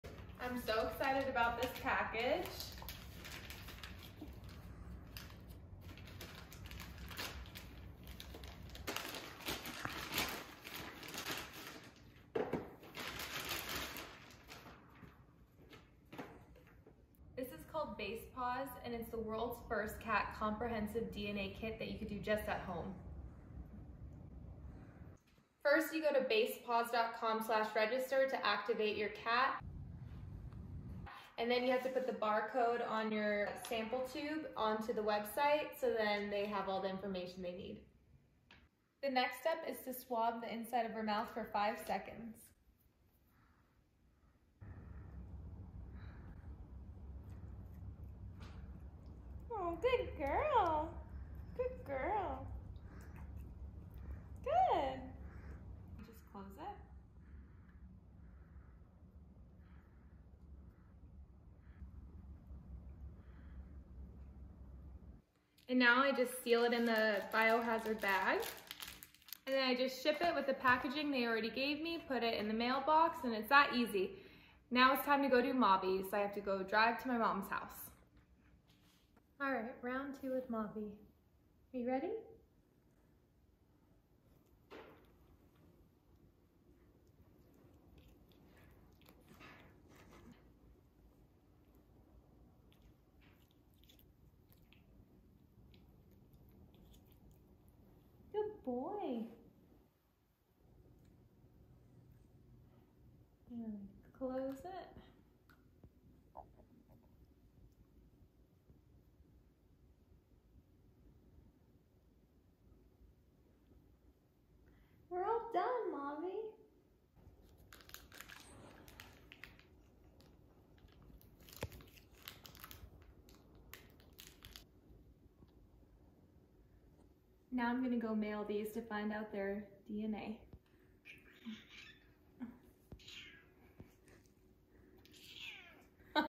How to DNA test your cats!
I am excited to show you both their DNA results, below is my discount I can provide for you so you can also find out more about your kitty!

Basepaws is amazing as it provides valuable genetic and health information about your cat!